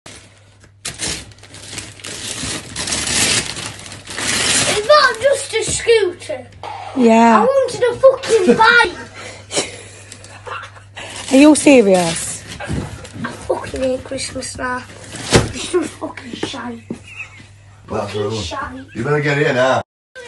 Kid wanted bike got a scooter
He wanted a bike and was not happy with just a scooter……. But he did get a bike

Please guys trying to grow my channel more. I appreciate it.